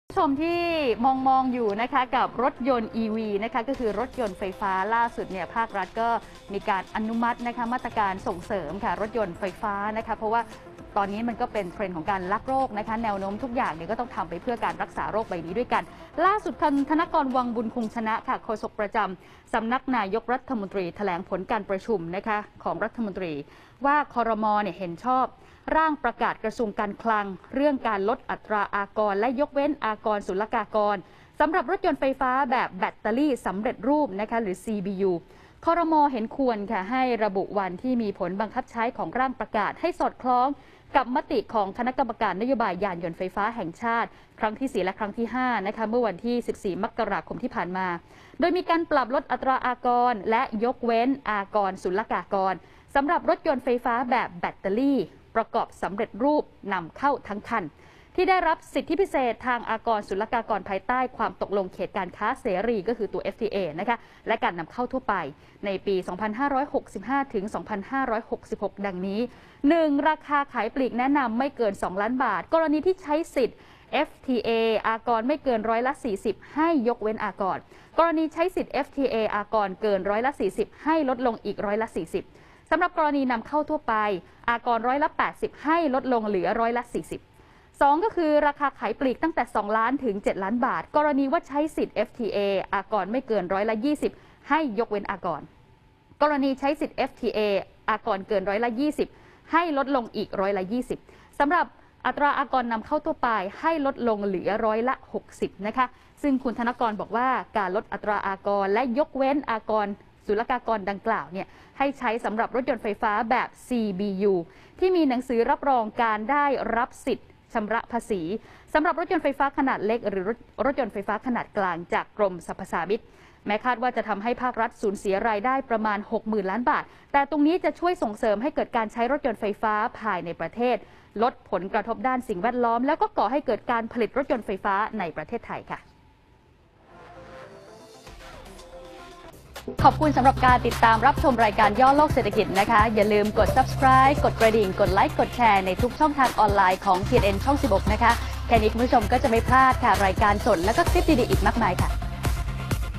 จัดเต็ม! ลดภาษีนำเข้ารถยนต์ EV สูงสุดร้อยละ 40 l ย่อโลกเศรษฐกิจ 22 ก.พ.65
ครม. ลดภาษีนำเข้าสำหรับรถยนต์ไฟฟ้าCBU ราคาขายปลีกไม่เกิน 2 ล้านบาท ลดสูงสุดร้อยละ 40 ขณะที่ราคาขายตั้งแต่ 2-7 ล้านบาท ลดลงสูงสุดร้อยละ 20 ช่วง 2 ปี 65-66 ยอมสูญเสียรายได้ 6 หมื่นล้าน